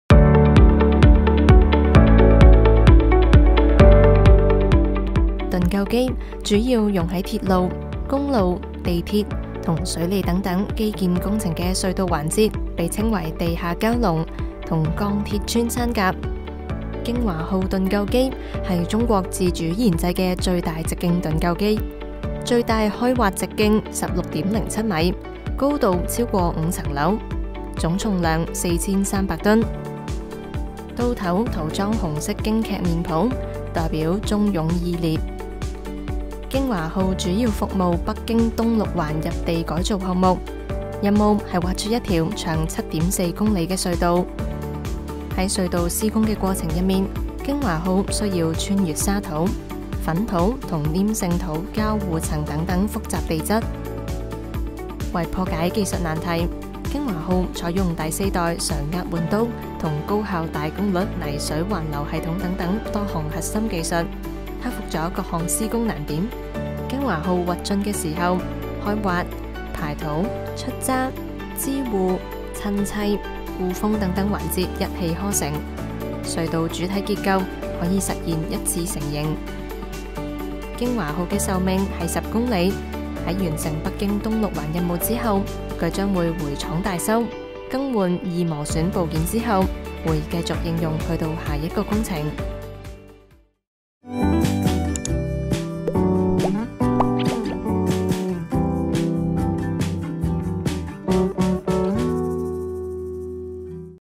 地下蛟龍「京華號」 中國超大直徑盾構機
盾構機，主要用於鐵路、公路、地鐵和水利等基建工程，猶如一條鋼鐵穿山甲，能在地下鑽出一條條隧道，既高效又安全，又被稱為「地下蛟龍」。

用於掘進北京「東六環改造工程」隧道的盾構機「京華號」，是中國自主研製的最大直徑泥水盾構機，究竟這個「吃土神器」是如何穿山挖道的？